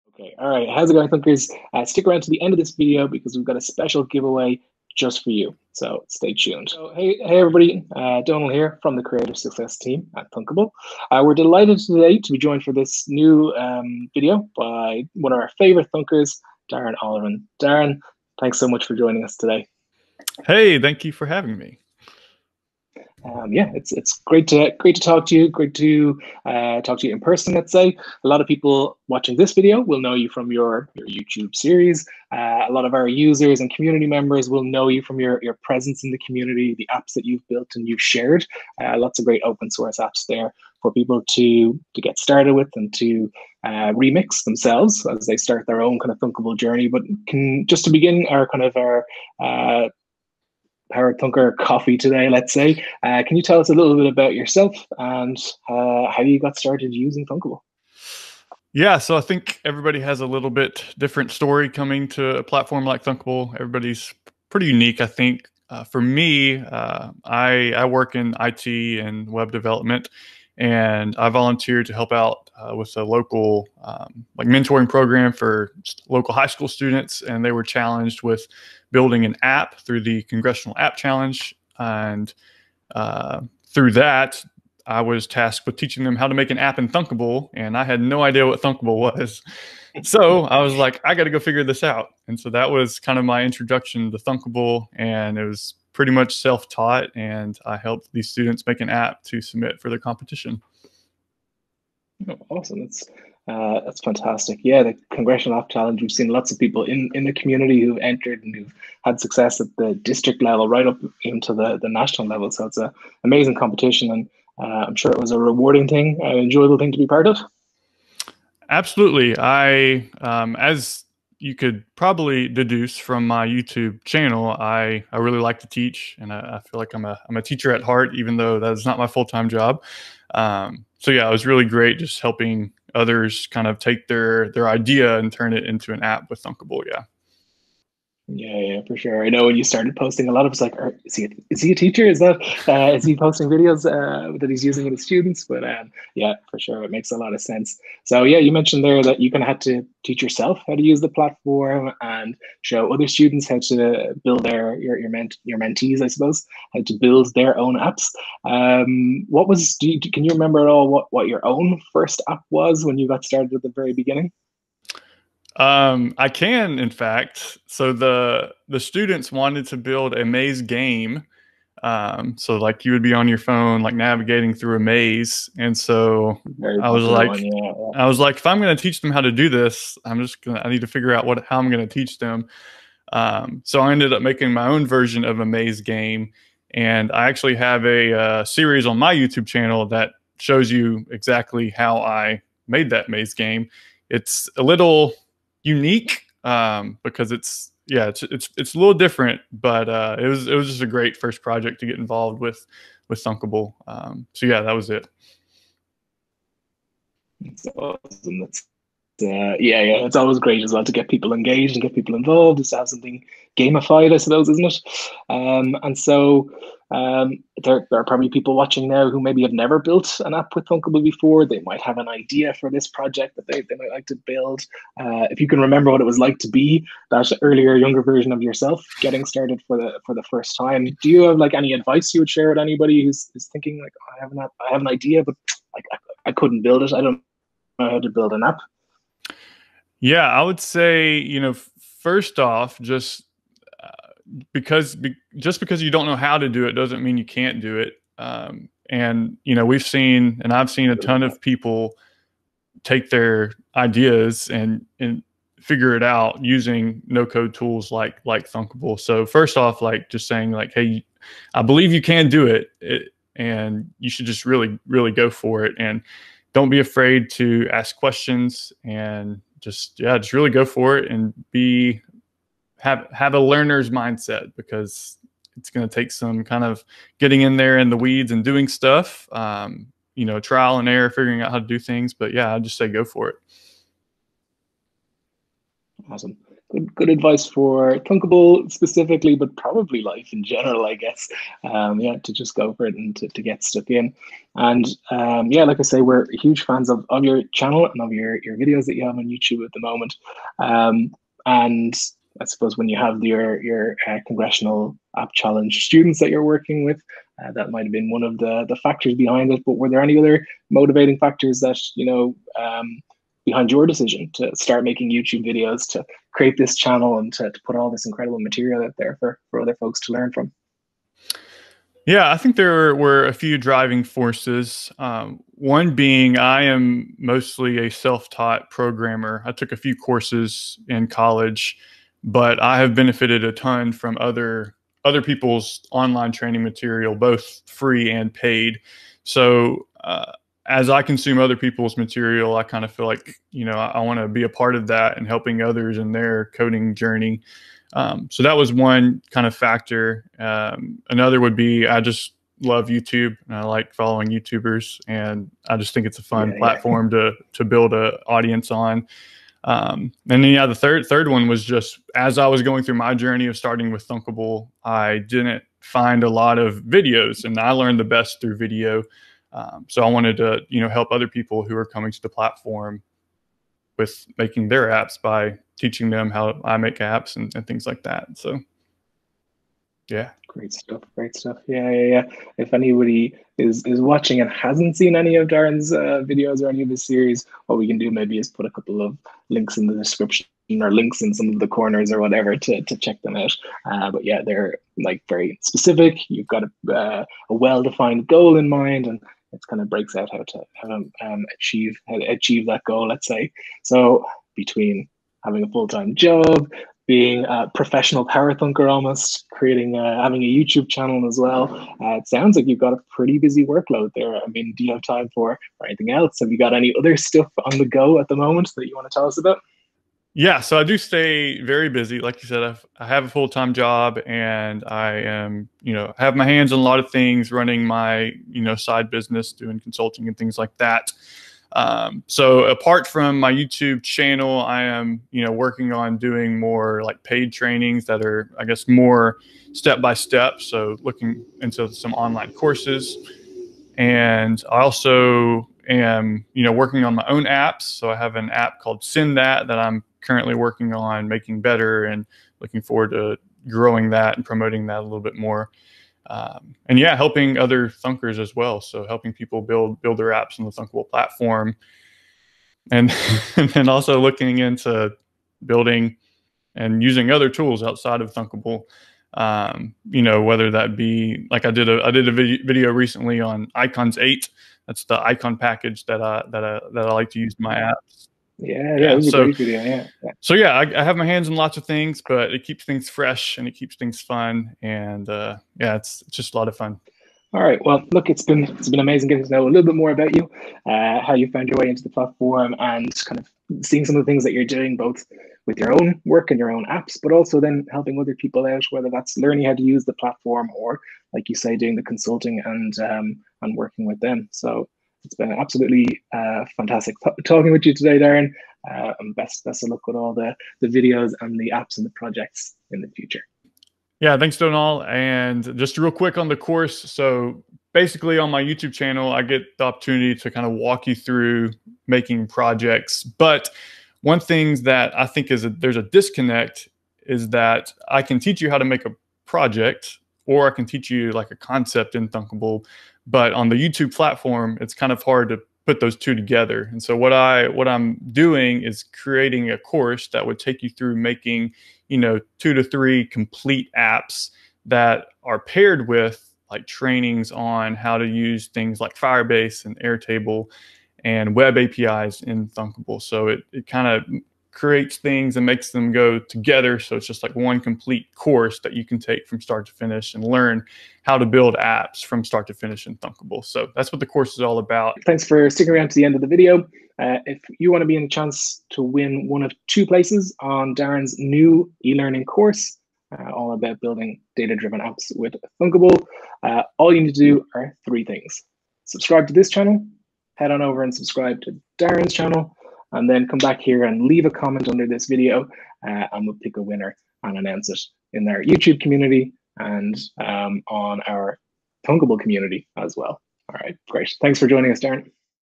Coffee with Thunkers: Darren
In these short 10 - 15 minute "coffee break" interviews we sit down with your favorite Thunkers from the Community to discuss all things App Development.